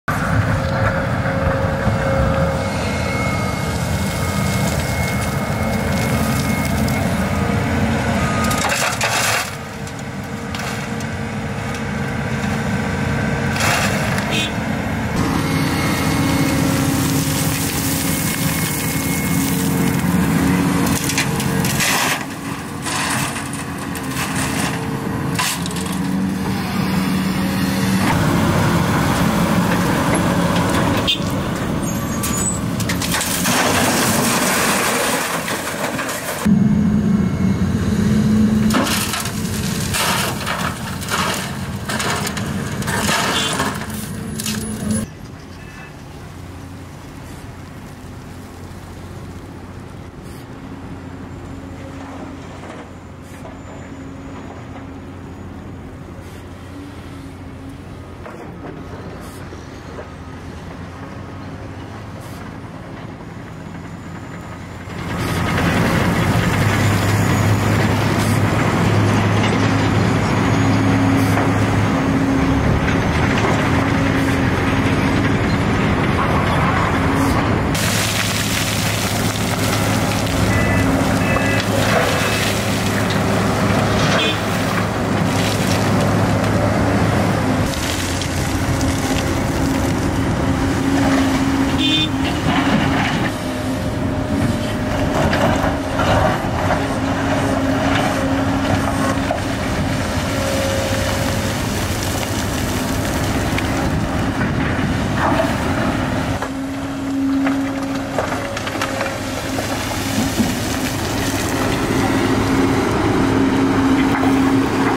Volvo 750 Excavator - RAW footage - Turner Mining Group
See the Volvo 750 in action in this RAW unedited video